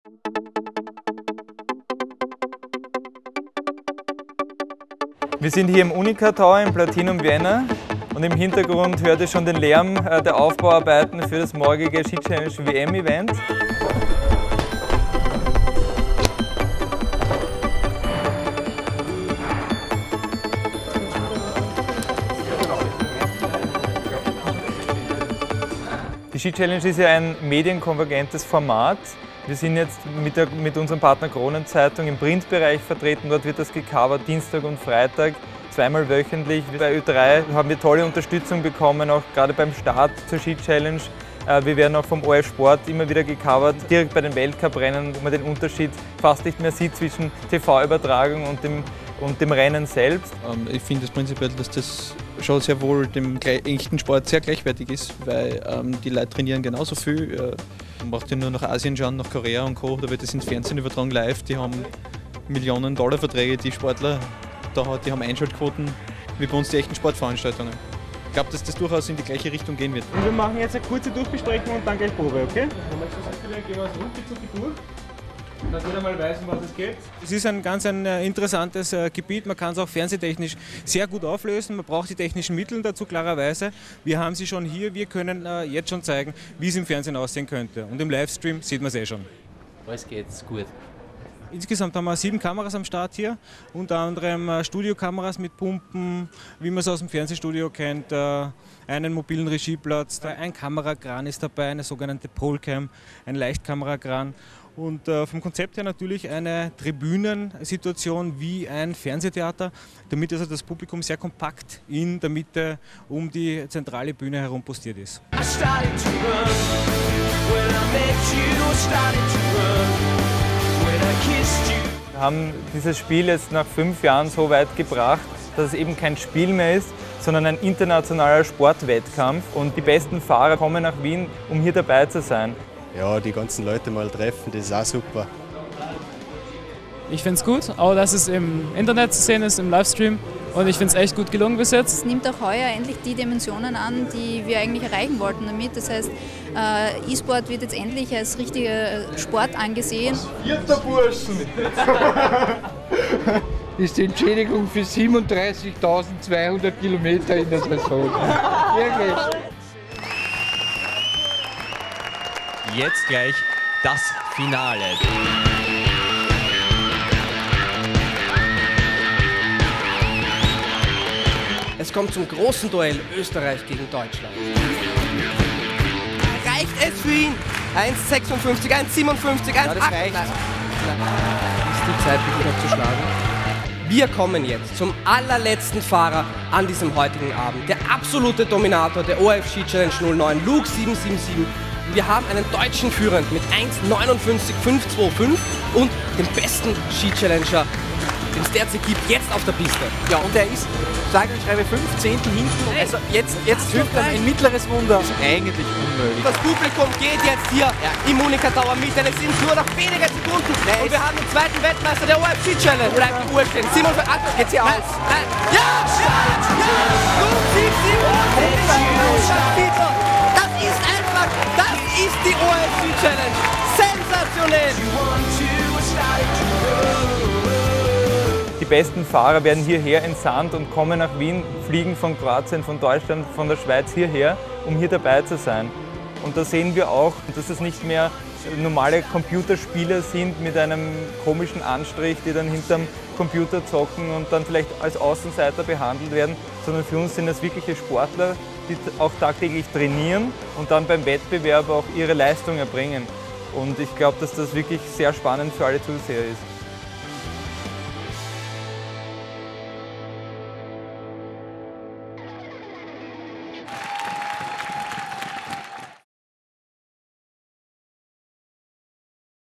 Making of ORF-Ski Challenge '09 Weltmeisterschaft
Ein exklusiver Blick hinter die Kulissen der SC:09-Weltmeisterschaft am 14. Februar 2009 im Uniqa Tower in Wien.